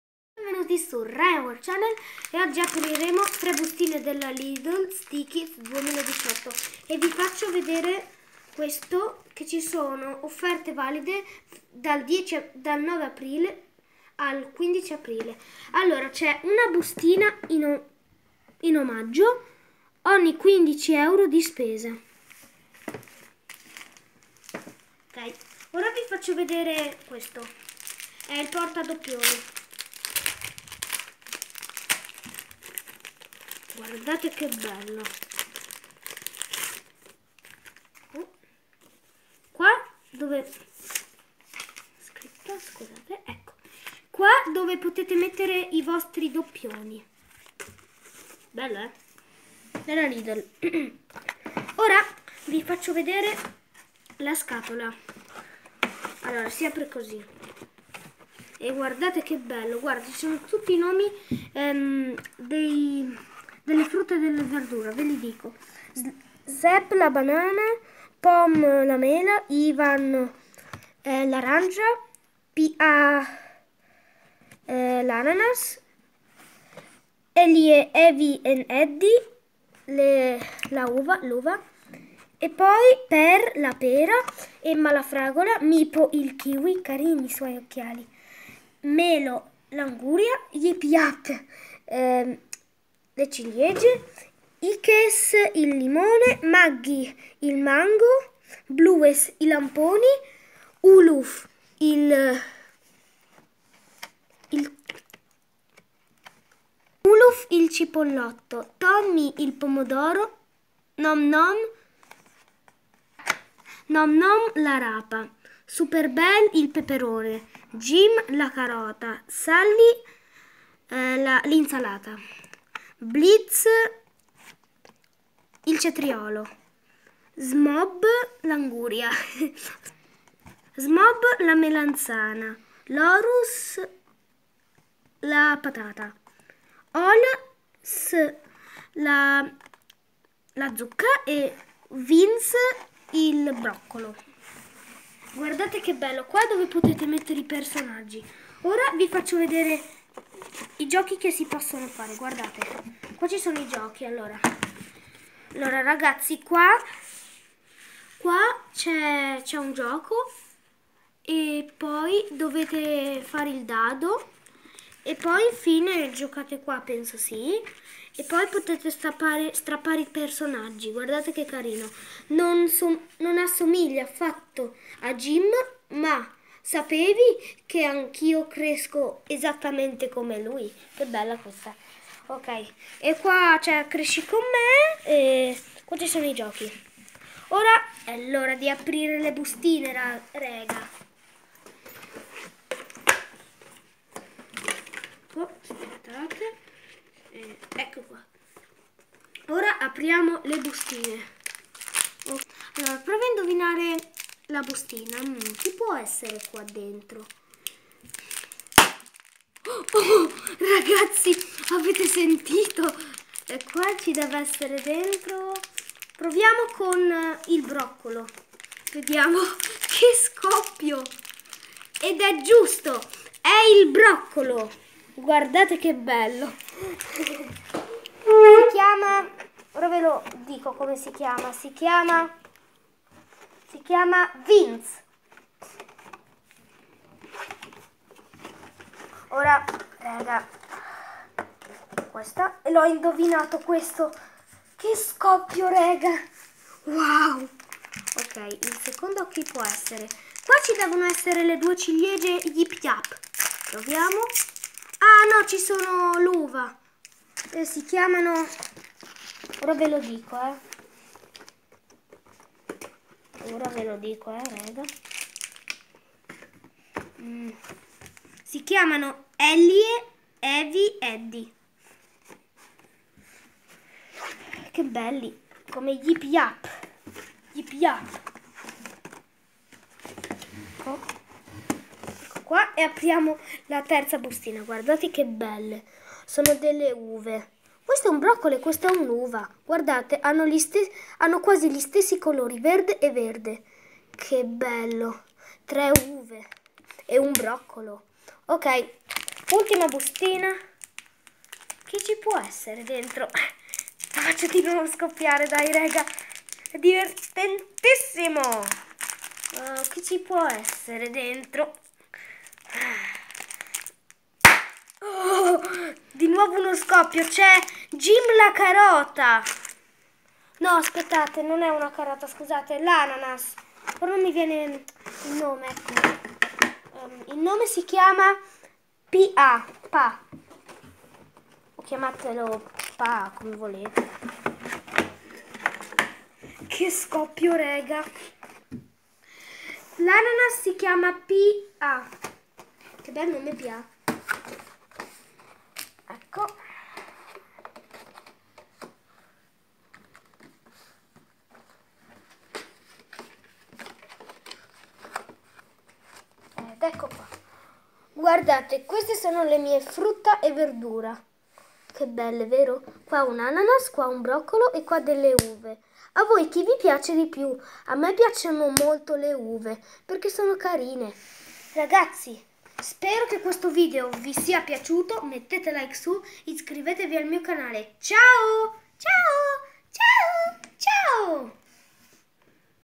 Stikeez Lidl 2018 Frutta & Verdura apriamo insieme 3 bustine
Nuova raccolta della Lidl 2018 stikeez frutta e verdura.
Contiene 24 stikeez che rappresentano